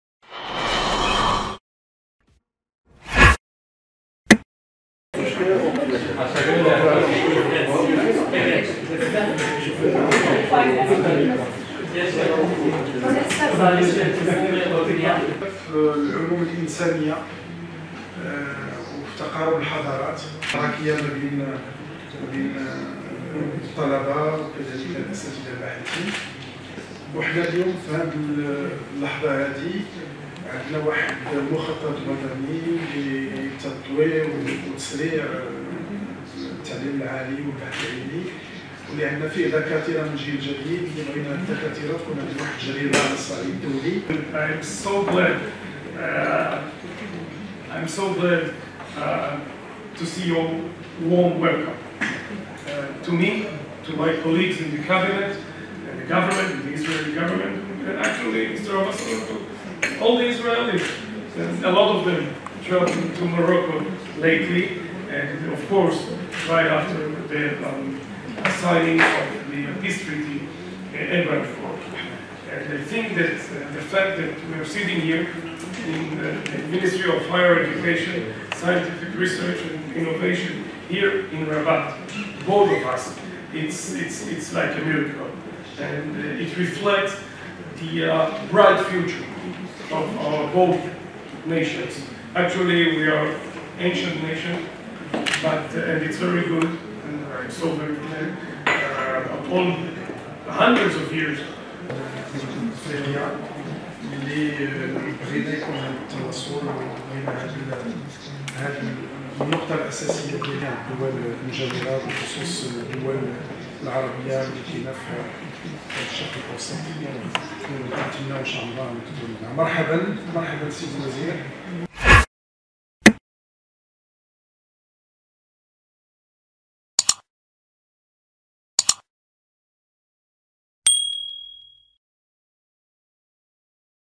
Israël se dite prête de coopérer le Maroc en matière d’innovation et de recherche scientifique
Israël par la voix de son ministre de l’Innovation, des sciences et de la technologie, Ofir Okunis qui entreprend depuis mardi 20 juion 2023 une visite officielle à Rabat, a exprimé sa disponibilité d’instaurer une coopération dense avec le Maroc dans les domaines de l’innovation et de la recherche scientifique.